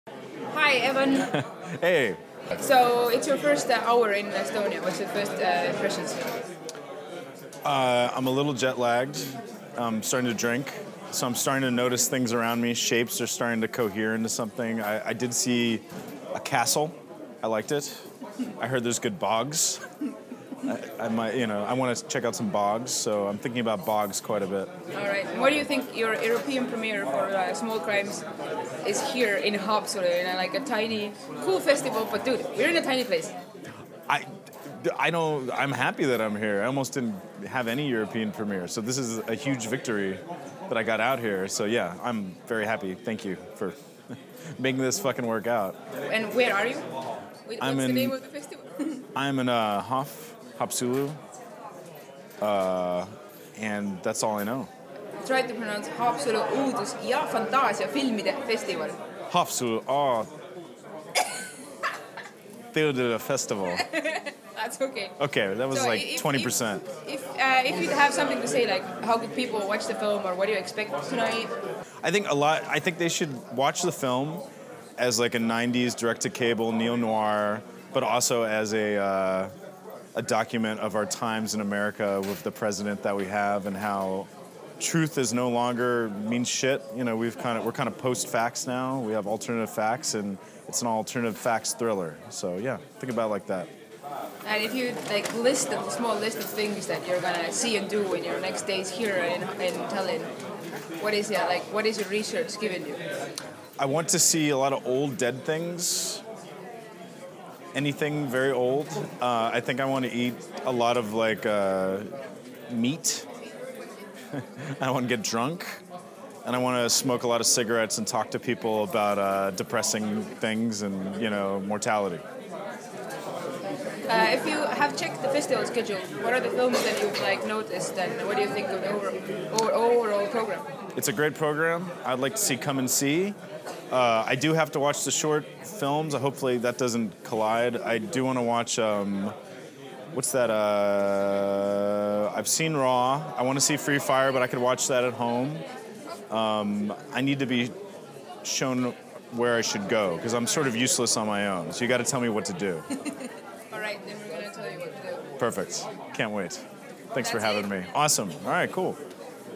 HÕFF 2017: Evan Katz
Interview with Evan Katz (Small Crimes, 2017) at the 12th Haapsalu Horror and Fantasy Film Festival (HÕFF).